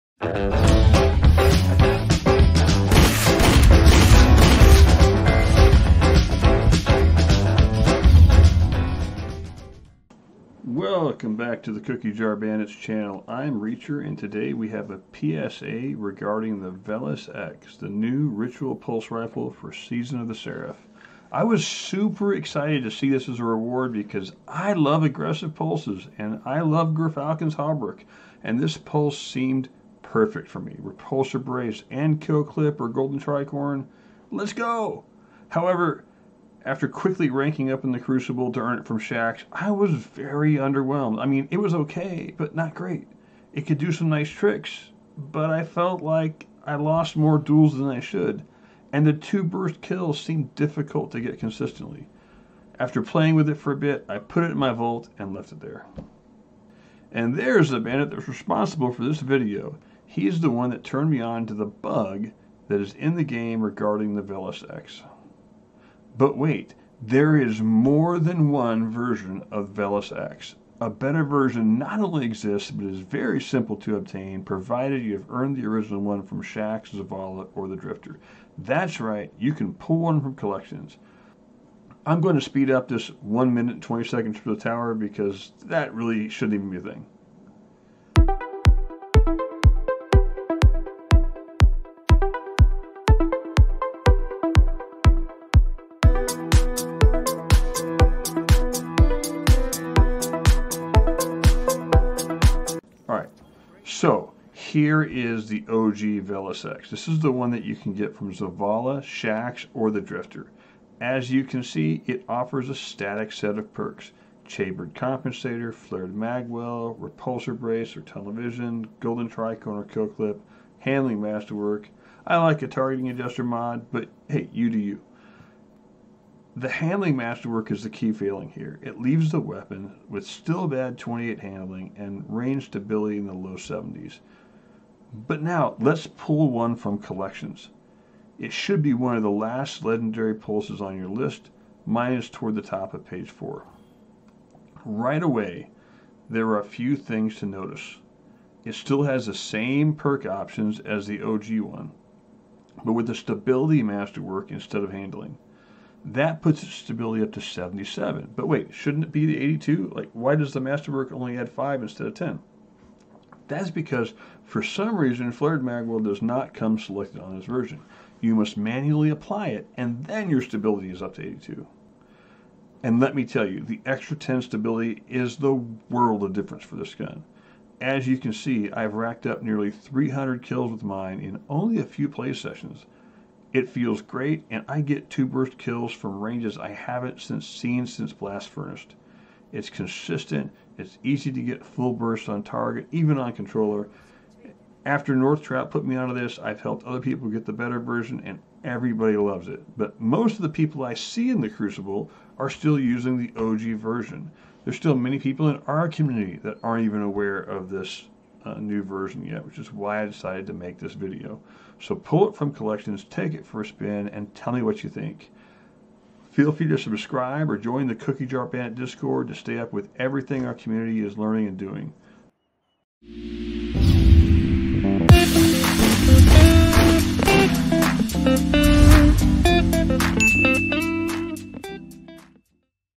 Bugged (And Better) Version of Veles-X!!!!! Get it now!!!
I know you saw Veles-X and wanted to swipe left. I get it. I don't blame you!  But did you know there's a better VERSION of it in the game? And nobody even seems to know it exists. I'll show you how to get it, and what it does.